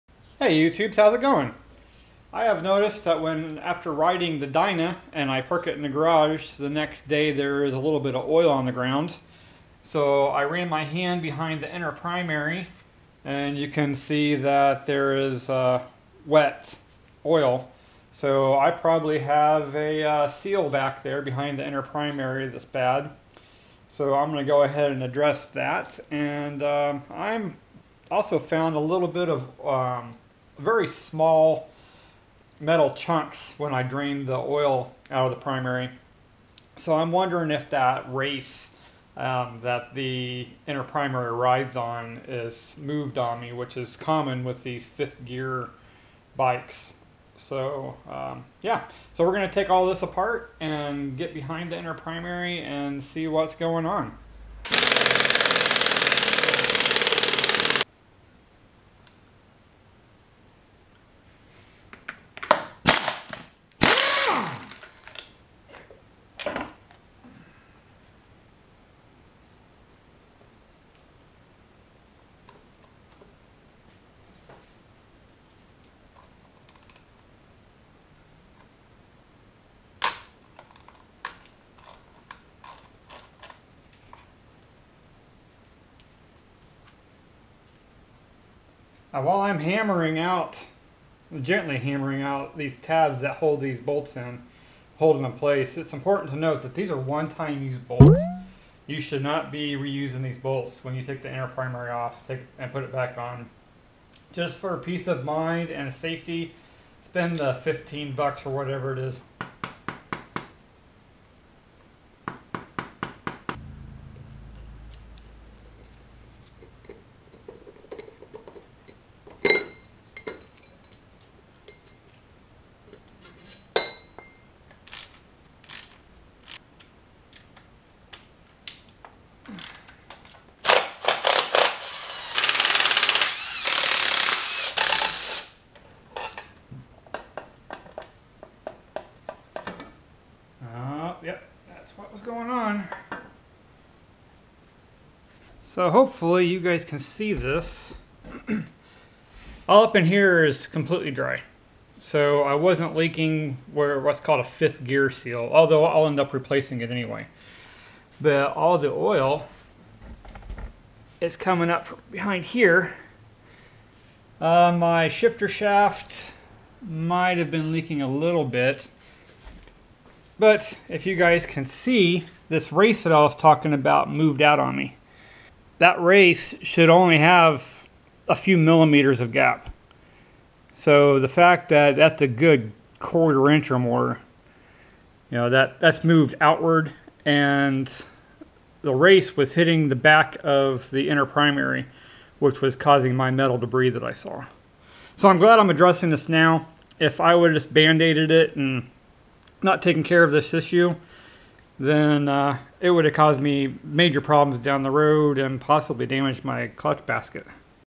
Fixing Harley Dyna Primary Leak
My primary is leaking oil and we find out why.  The following video will be the fix...